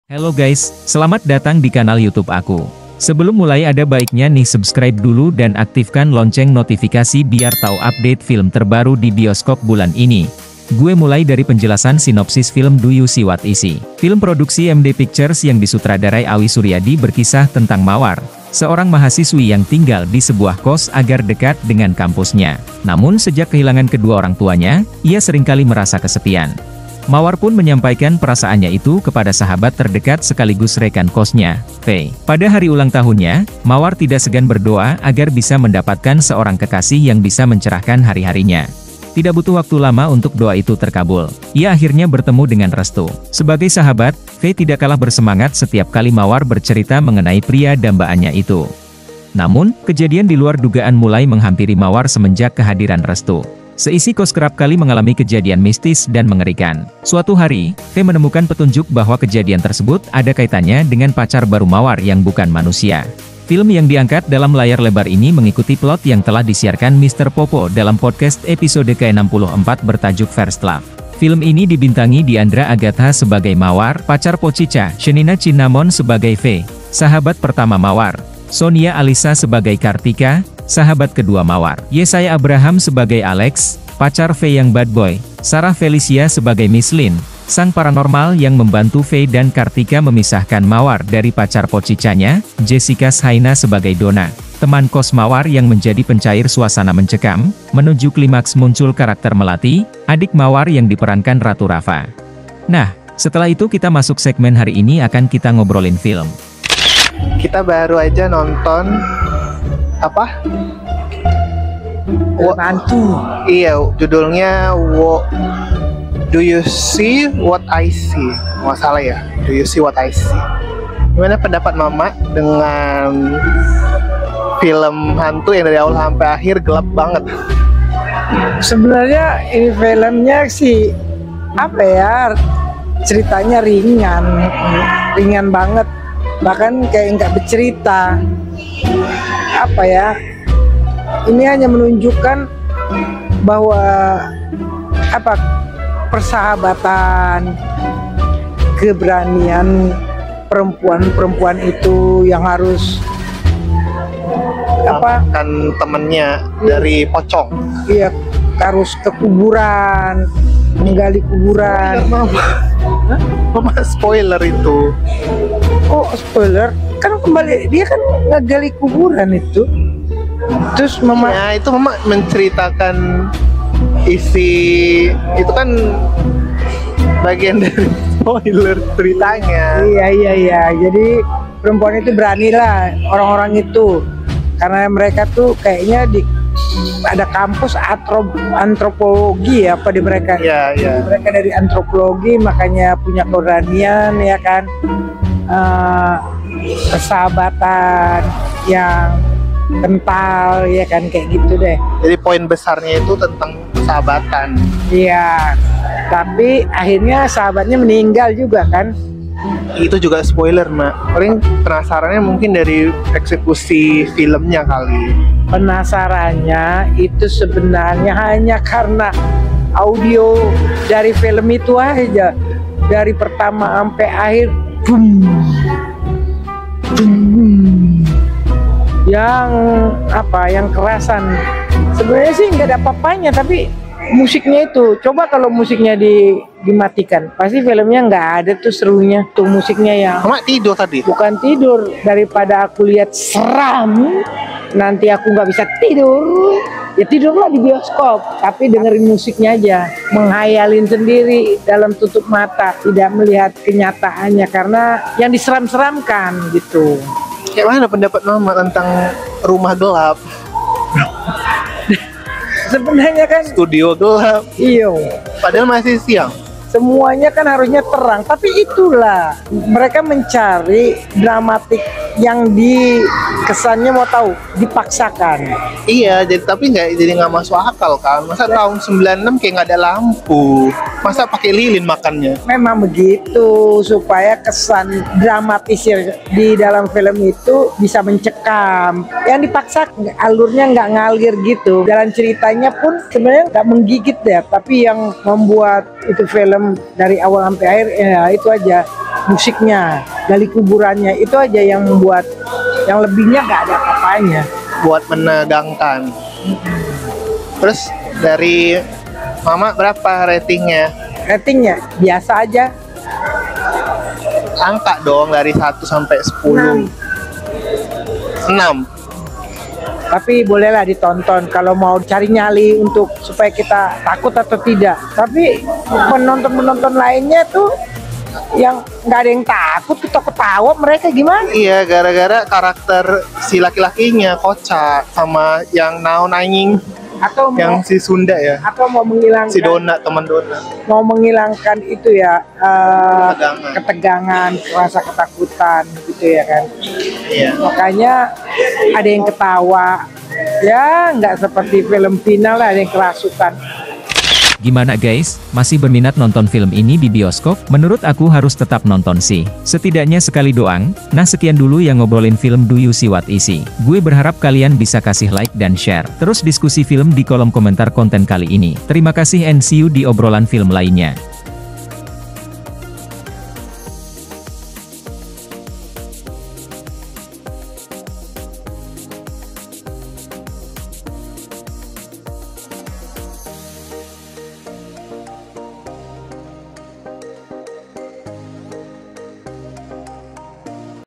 DO YOU SEE WHAT I SEE FIRST LOVE - DIANDRA AGATHA, SHENINA CINNAMON | SINOPSIS DAN REVIEW FILM
Sudah mampir?
terima kasih sudah nonton video "HARI INI KITA NGOBROLIN FILM DO YOU SEE WHAT I SEE FIRST LOVE | DIANDRA AGATHA, SHENINA CINNAMON" sampai habis.
Tinggalkan jejak dong!
Dilike kek,
Dikomen kek,
Disubscribe kek,
Dishare kek